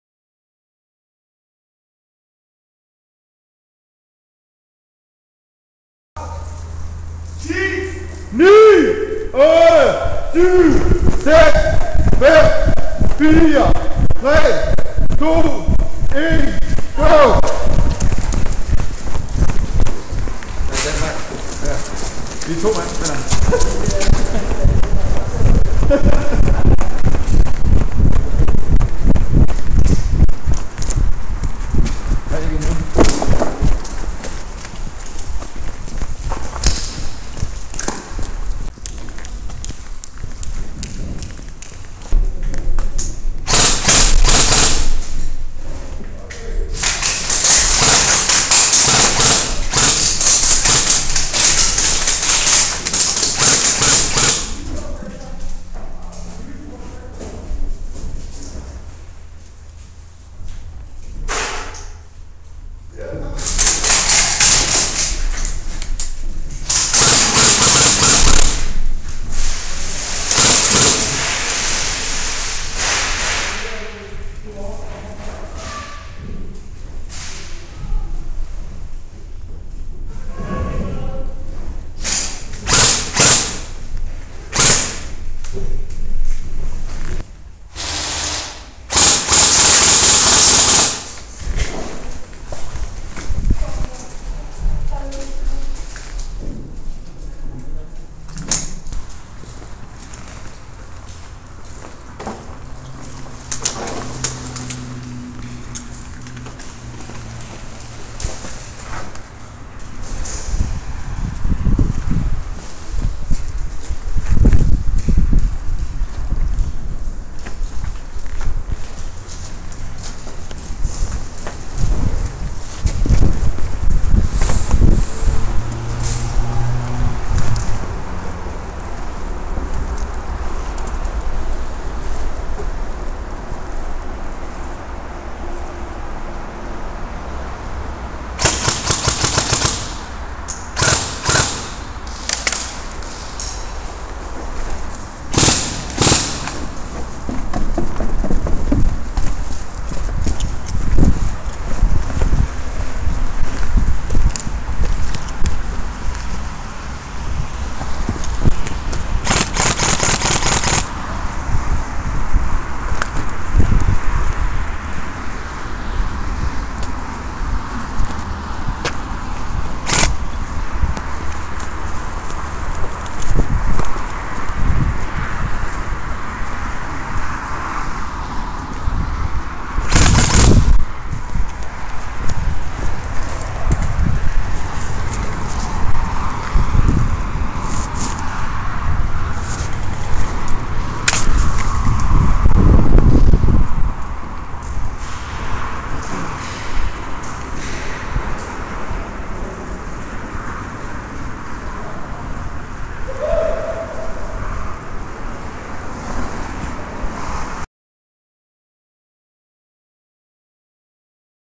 Hardballspil på sjælland
Optaget med V.I.O. hjelm kamera

Riffel : Systema PTW CQB